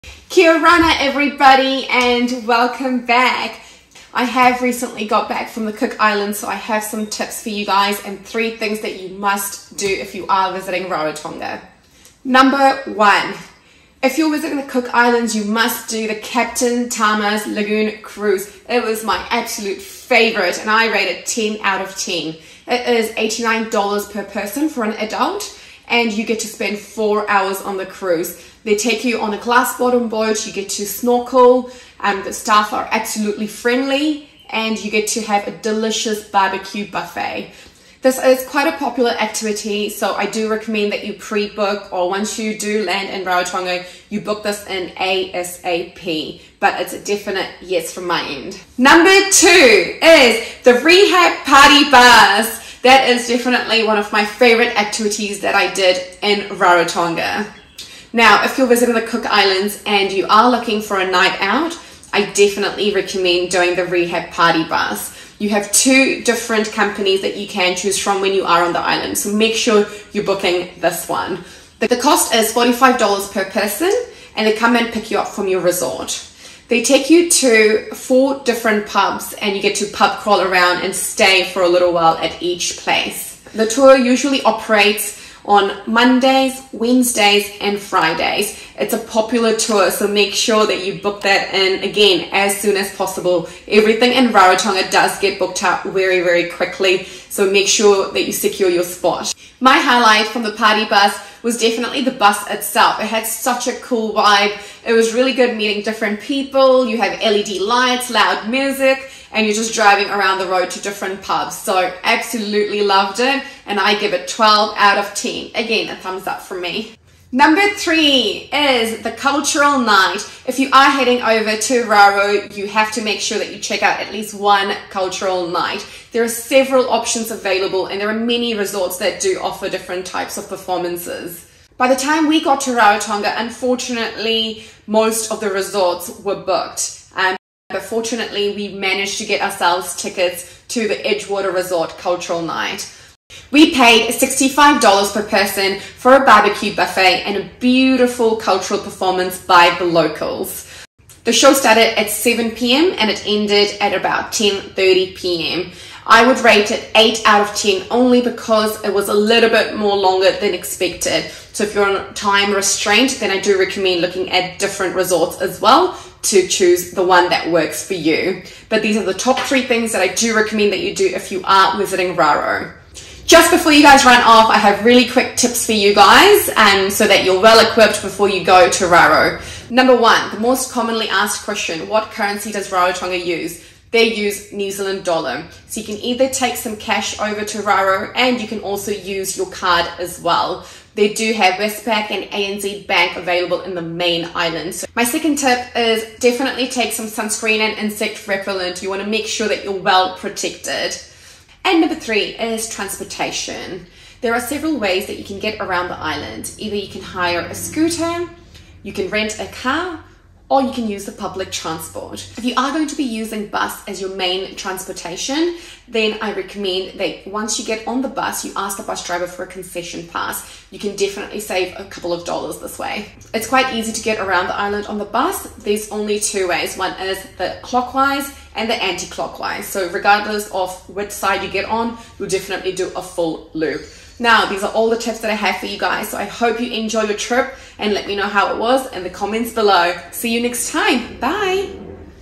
TOP 3 ACTIVITIES YOU MUST DO IN RAROTONGA, COOK ISLANDS | TRAVEL TIPS 2022
Useful information if you're heading over to Rarotonga. Don't forget to watch until the end for some travel tips!